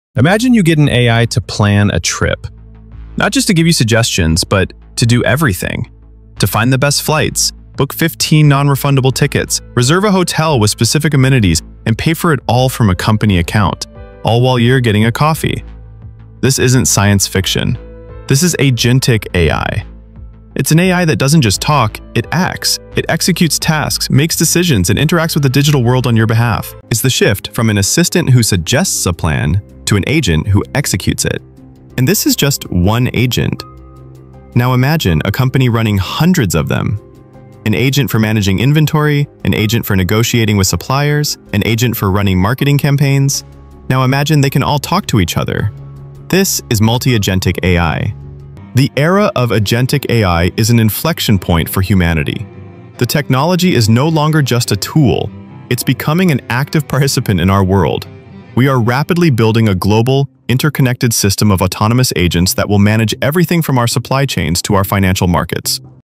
Is This the Smartest AI Ever Made?
Explore the power of Agentic AI, an AI that goes beyond simple suggestions to autonomously execute tasks and make decisions, revolutionizing AI Automation. This cutting-edge AI can handle complex operations like booking flights and managing finances, showing the shift towards Autonomous Systems. Discover how this tech is changing the AI Workflow.